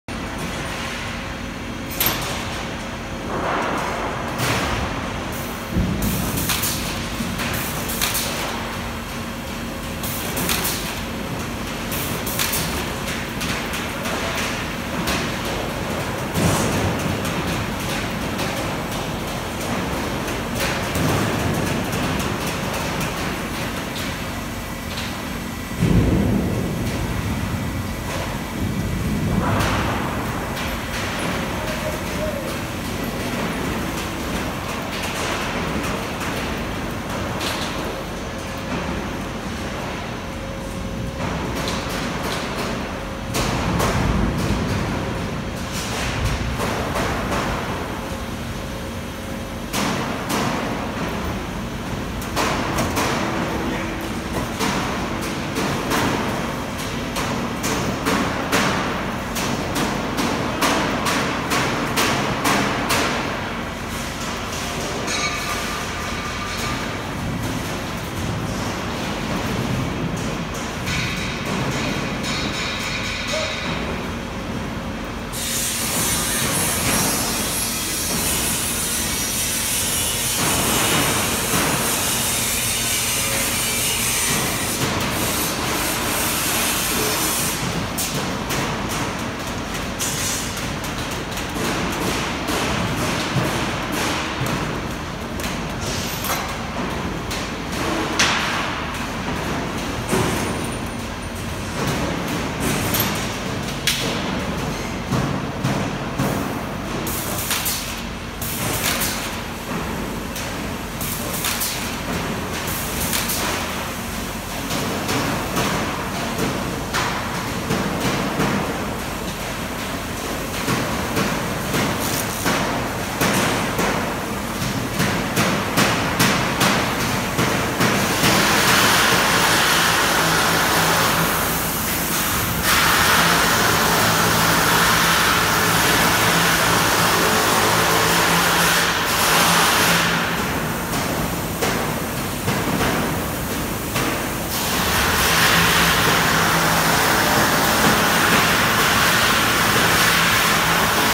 Pre-Owned Iowa Precision 6’ Right Hand “U” Full Duct Line installed in 2000
4 Coil Stations with Grid Auto Feed System
Notching for S&D and TDC
Tie Rod Hole Punch
Male/Female Duplex Rollformer
Drive Cleat Roller
TDC Duplex
Insulmatic with Extruded Glue System & Edge Trim Slitter
Duro Dyne 5 Head Pinspotter
FULL  Wrap Brake
If you MUST have new controls, it runs fine as it is now, but if you need the latest controls this line would need to be retrofitted with New Windows Based Control System which normally runs aroun $55-$65,000+/-

1.      IPI Bosch Hydraulic Servo Valve Package
2.      Windows 10 based solid-state touch screen control
3.      Laptop computer with complete IPI coil line program to be used as system backup
4.      New Encoders with their respective cables to replace existing units

CALL FOR PHOTOS & ADDITIONAL DETAILS & PRICE. Serious inquiries only please, this line is available for immediate purchase with shipment in late June. Call Norm